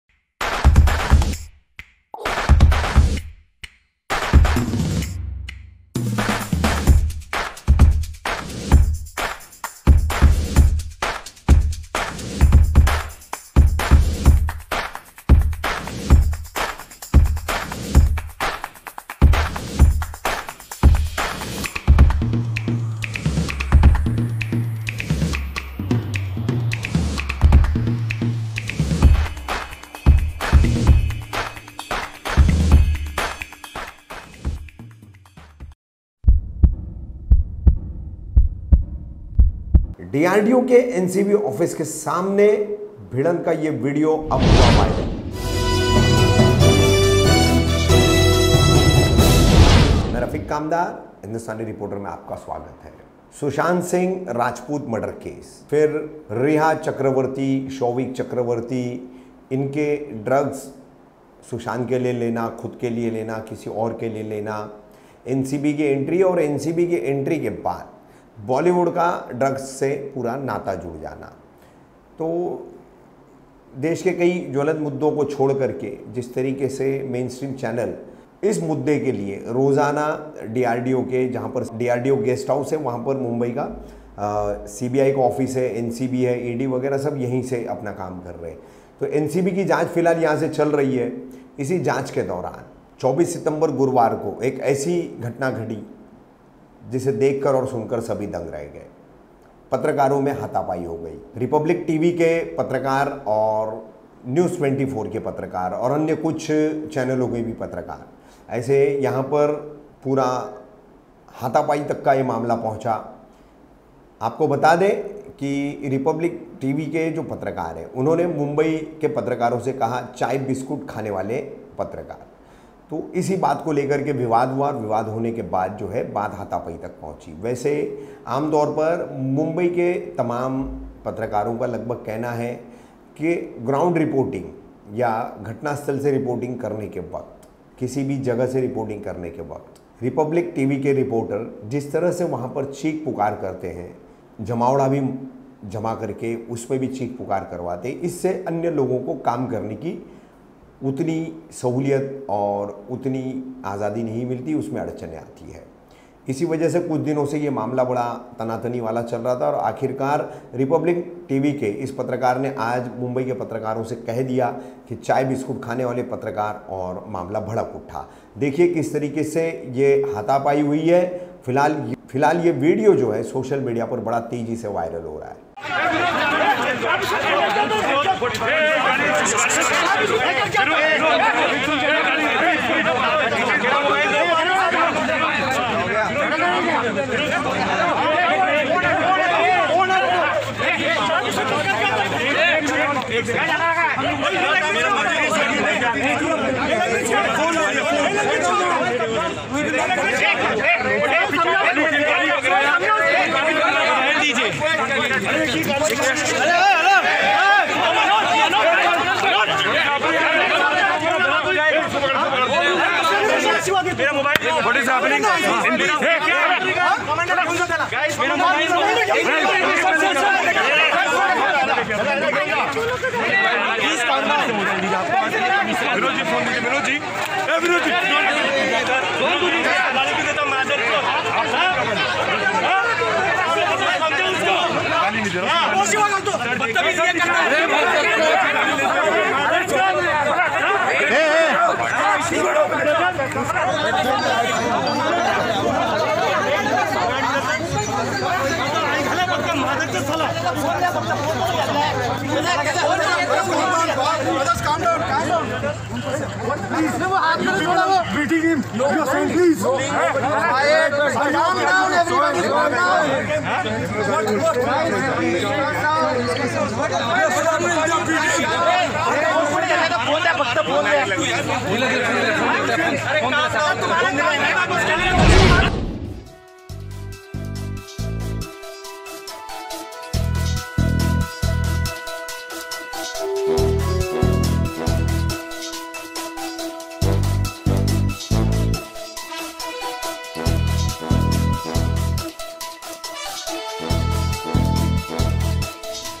रिपब्लिक टीवी के पत्रकार और अन्य पत्रकारों में हुई भिड़ंत | Hindustani Reporter
रिपब्लिक टीवी के पत्रकार और अन्य पत्रकारों में हुई भिड़ंत

Mic We Use